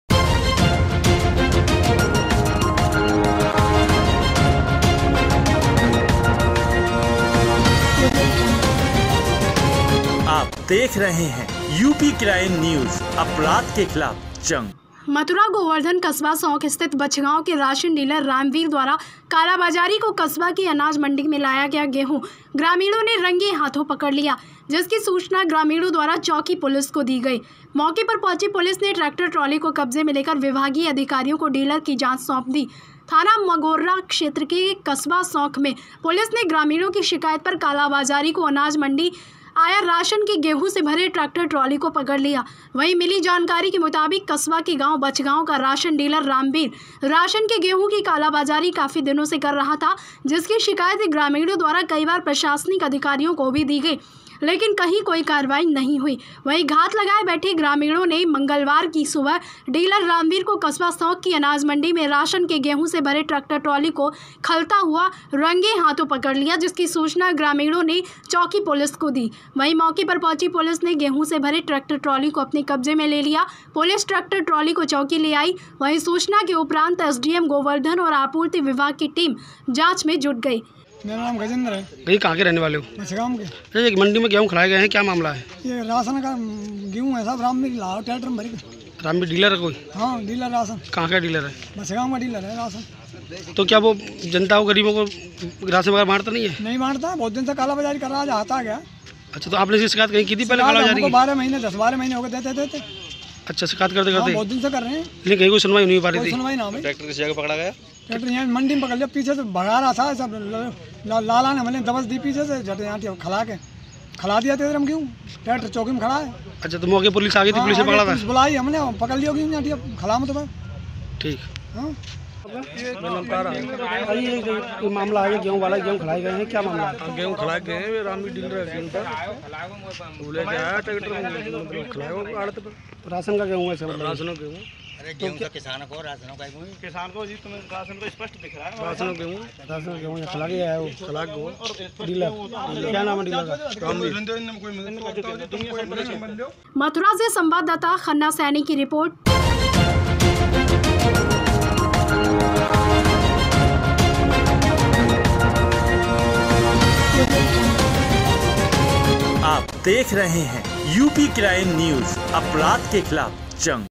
मथुरा-आखिरकार कालाबाजारी को आया गेहूं पकड़ा ही
कस्बा सौंख स्थित बछगांव के राशन डीलर रामवीर द्वारा कालाबाजारी को कस्बा की अनाज मंडी में लाया गया गेहूं  ग्रामीणों ने रंगे हाथ पकड लिया. जिसकी सूचना ग्रामीणों द्वारा चौकी पुलिस को दी गई मौके पर पहुंची पुलिस ने  ट्रैक्टर ट्रॉली को  कब्जे में लेकर  विभागीय अधिकारियों को डीलर की जांच सौंप दी ।
थाना मगोर्रा क्षेत्र के कस्बा सौंख में पुलिस ने ग्रामीणों की शिकायत पर कालाबाजारी को अनाज मंडी आया राशन के गेहूं से भरे ट्रैक्टर ट्रॉली को पकड़ लिया. प्राप्त जानकारी के अनुसार कस्बा के गांव बछगांव का राशन डीलर रामवीर राशन के गेहूं की कालाबाजारी काफी दिनों से कर रहा था. जिसकी शिकायत ग्रामीणों द्वारा कई बार प्रशासनिक अधिकारियों को भी दी गई. लेकिन कहीं कोई कार्यवाही नहीं हुई. वही घात लगाए ग्रामीणों ने मंगलवार की सुबह डीलर रामवीर को कस्बा सौंख की अनाज मंडी में राशन के गेहूं से भरे ट्रैक्टर ट्रॉली को खलता हुआ रंगे हाथ पकड़ लिया. जिसकी सूचना ग्रामीणों द्वारा चौकी पुलिस को दी गई. वहीं मौके पर पहुंची पुलिस ने गेहूं से भरे ट्रैक्टर ट्रॉली को अपने कब्जे में ले लिया. पुलिस ट्रैक्टर ट्रॉली को चौकी ले आई. वही सूचना के उपरांत एसडीएम गोवर्धन व आपूर्ति विभाग की टीम जांच में जुट गई.